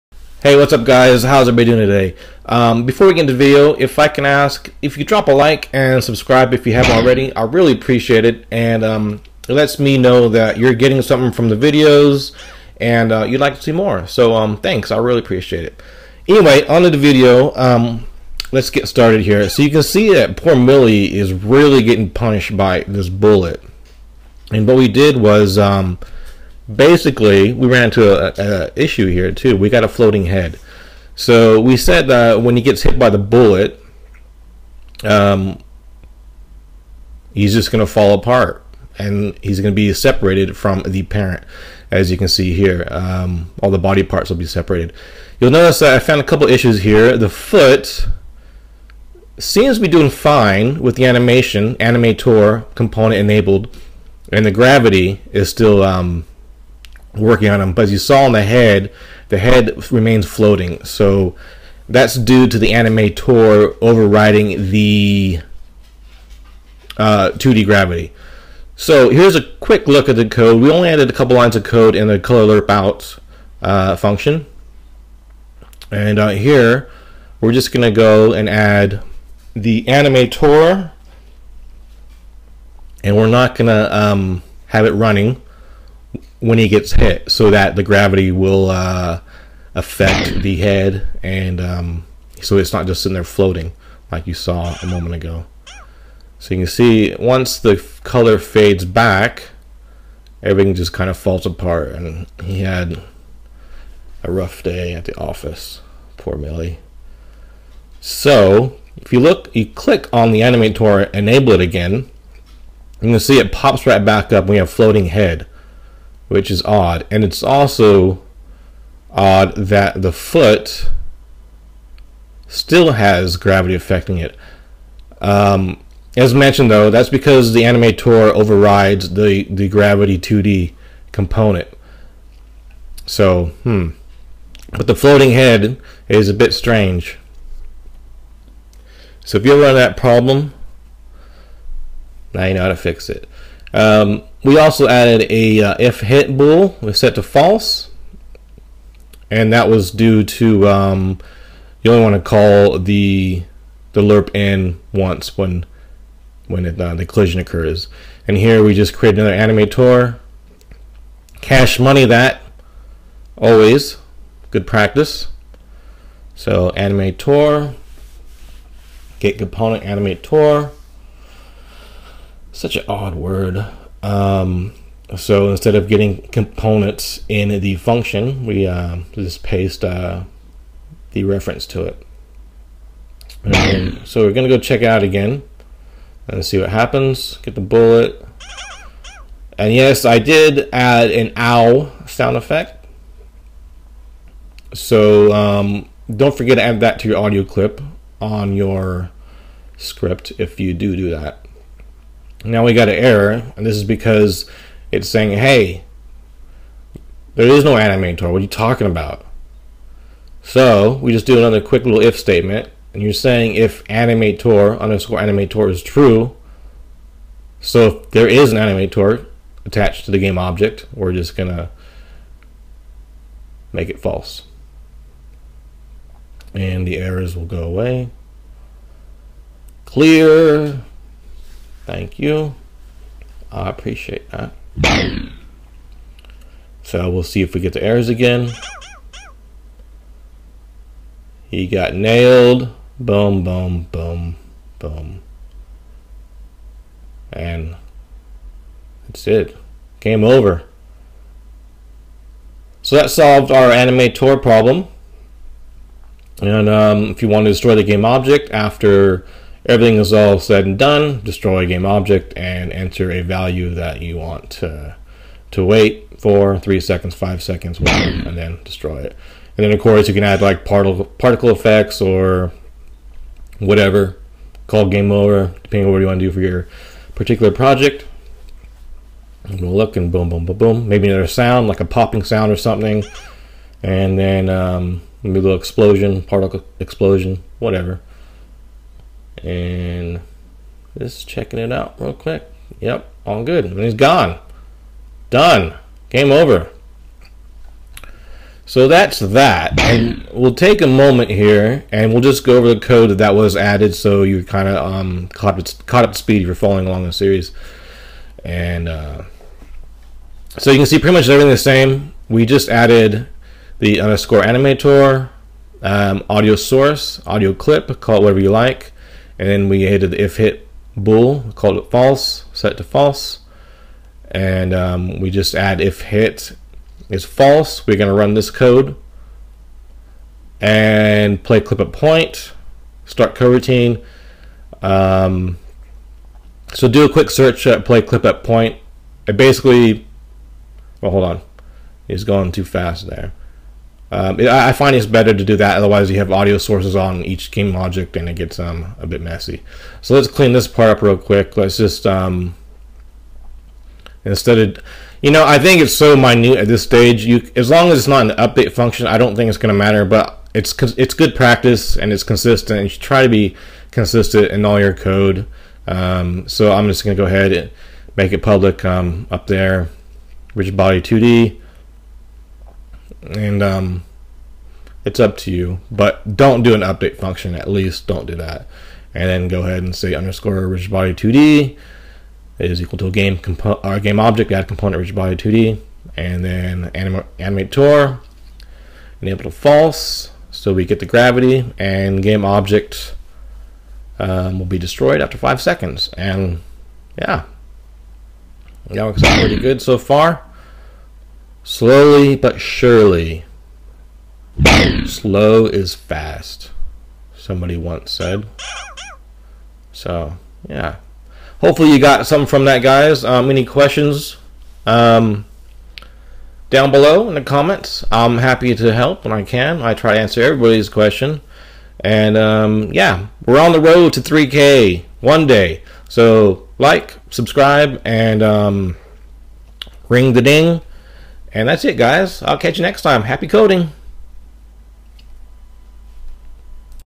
#7 Rigidbody2D Not Falling Anymore | Animator Overrides | Unity Beginner Tutorial
The Animator Component Overrides the Rigidbody2D as Shown in the Example.  We Added some More Variables to our checkCollision Script.  They are:  public Animator _animator, public AudioSource _aS, public AudioClip _acOw, public bool ifHit=false, and public Rigidbody2D _rigidBody2d.

In the OnCollisionEnter2D function we added ifHit so that we only call colorLerpIn one time.  In the colorLerpOut function we added transform.parent=null so that our sprites will no longer be children of the parent gameObject.

We then add Rigidbody2D dynamically at runtime so that our sprites are affected by gravity being hit by the bullet.

Next, we check to see if the sprite has an Animator component and if it does we set the Animator to false.

Finally we Destroy the gameObject!

Happy Coding!